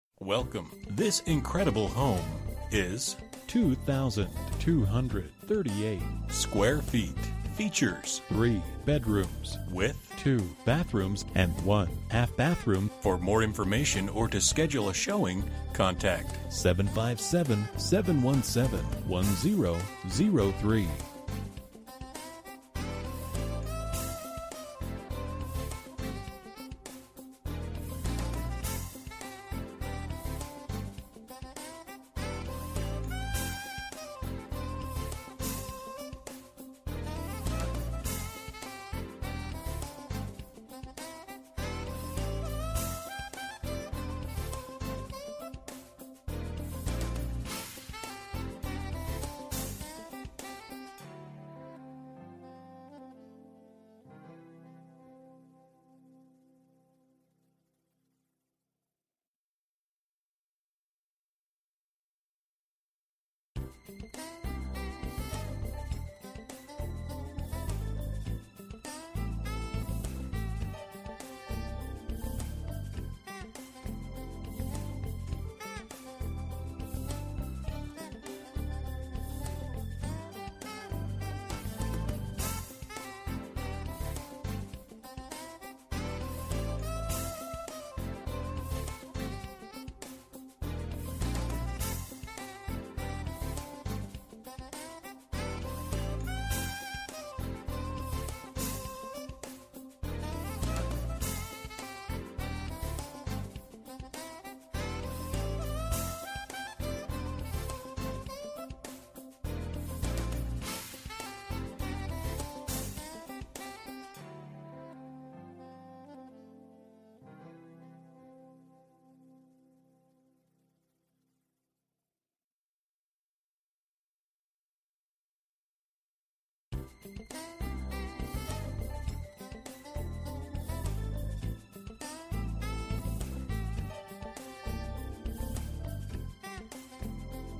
Property for Sale - 30 S BOXWOOD ST, Hampton, VA 23669
BANK APPROVED!! Old Hampton charm offers over 2200sqft! Huge Kitchen; formal family room & dining area; updated baths including a double sink vanity in spacious master bedroom. Walk to downtown.

Bedrooms: 3
Bathrooms: 2 full and 1 half Bathrooms
Square Feet: 2238
Price: $78,000